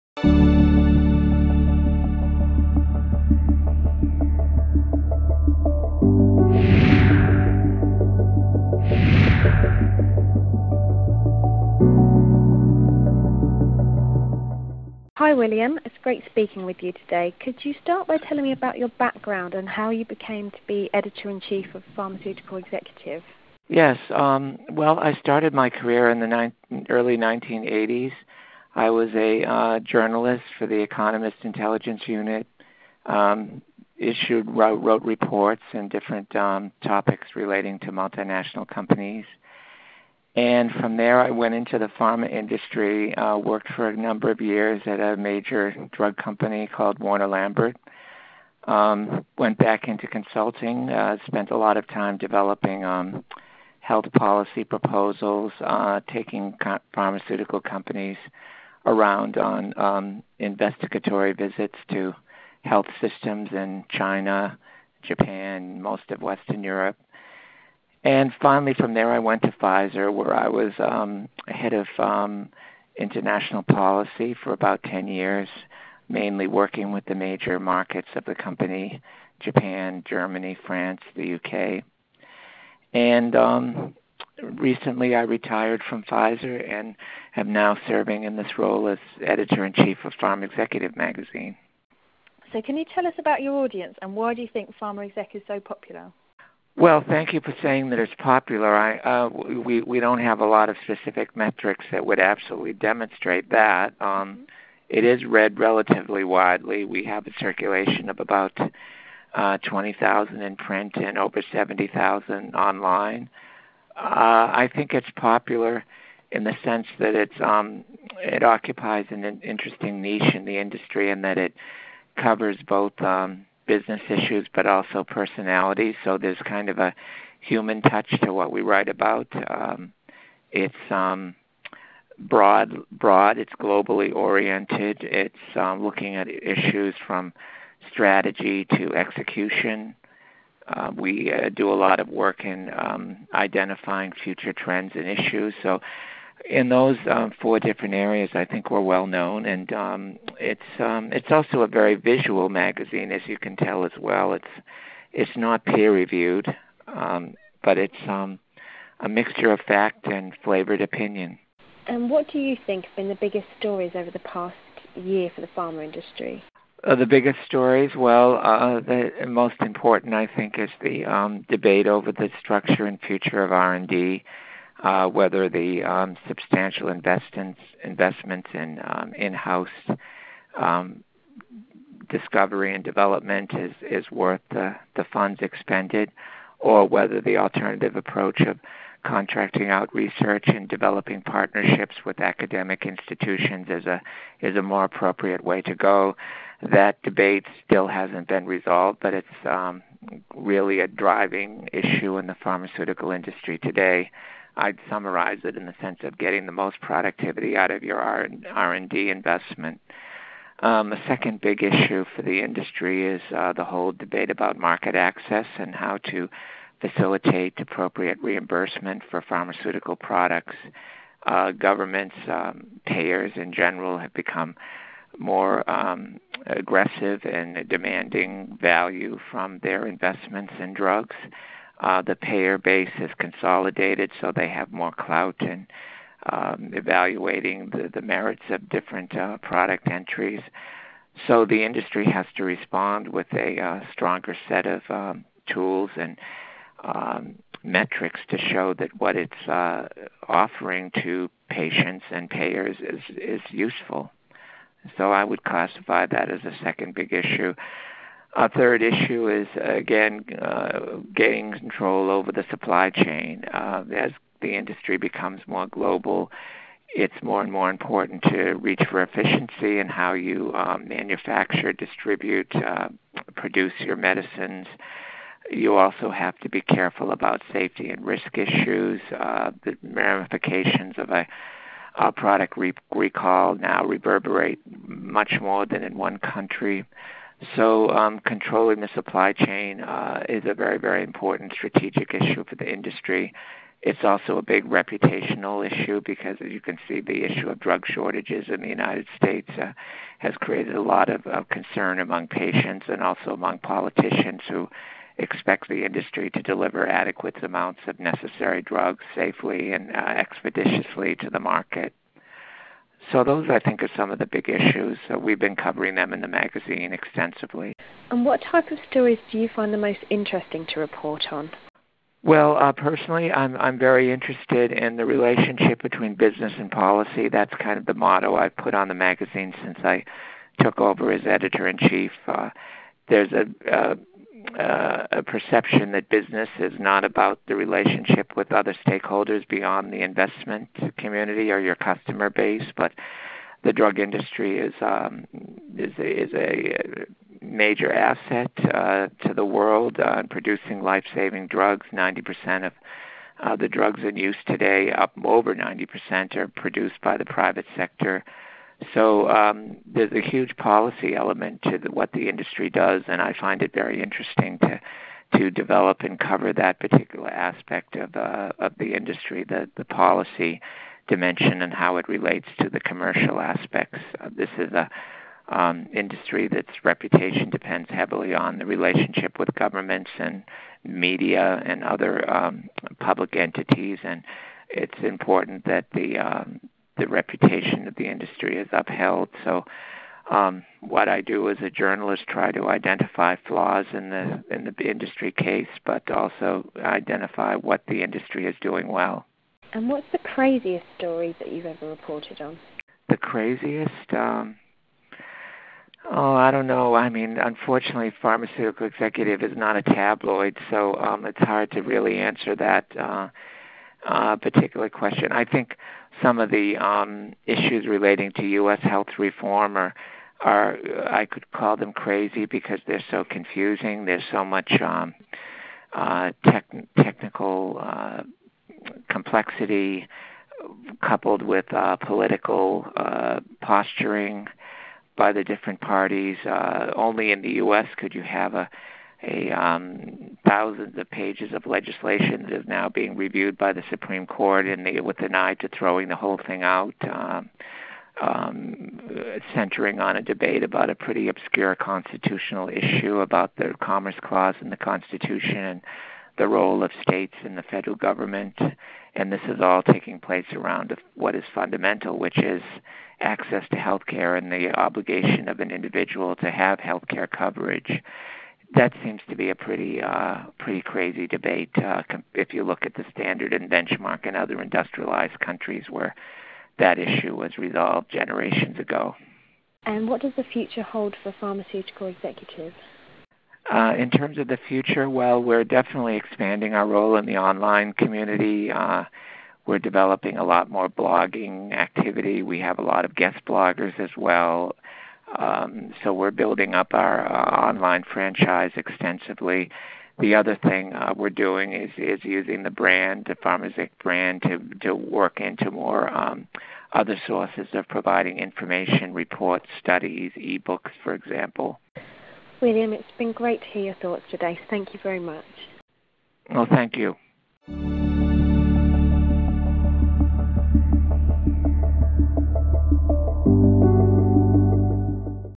Media perspectives: William Looney
For the supporting article and to add your comments please

In our latest Media Perspectives interview, we speak to William Looney from Pharmaceutical Executive about his highlights as Editor-In-Chief and his thoughts on some of this year's top pharma news stories.

00:15 -- William talks about his background and his current role
01:32 --Pharma Exec's audience and popularity
02:41 -- Discussion around some of the biggest pharma stories from the past year
05:18 --The type of stories William is most interested reporting about
06:39 -- Some of Pharma Exec's craziest stories
07:59 -- A look towards the future of the magazine